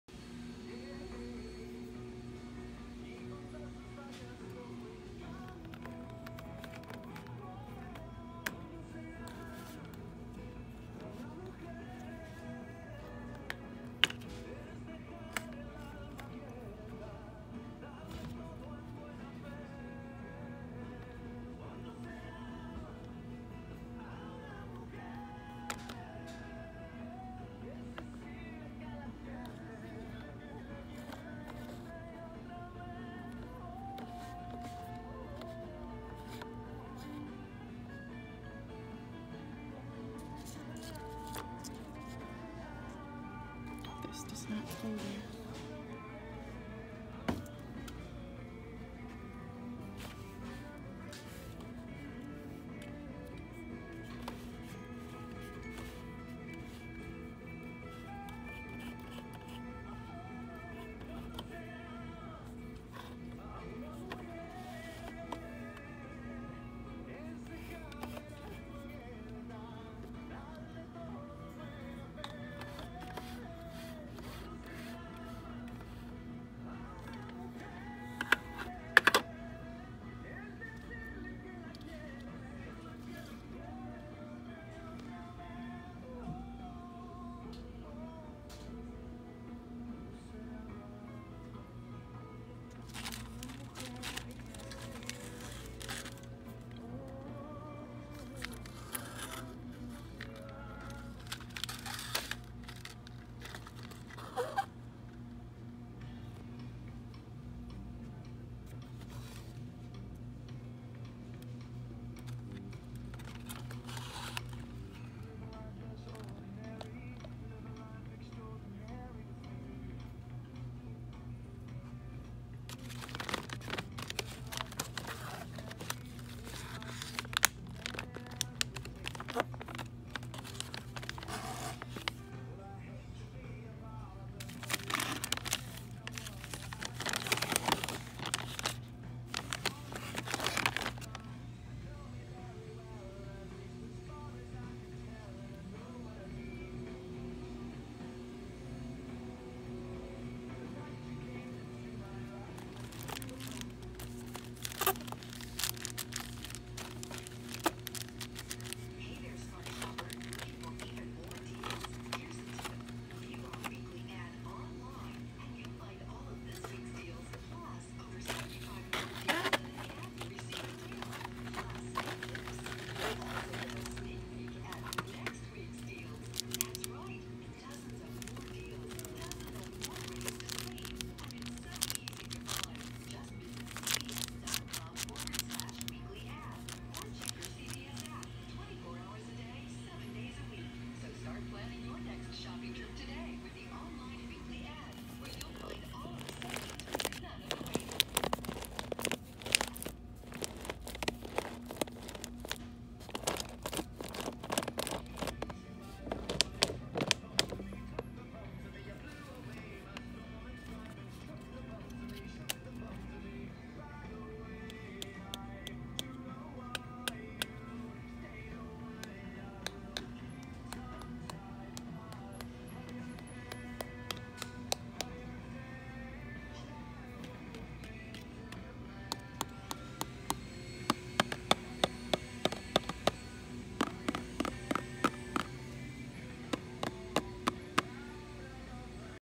ASMR CVS Cat Food Aisle, Straightening Cans
I had to do it! Lotsa cans were outta order, but not anymore.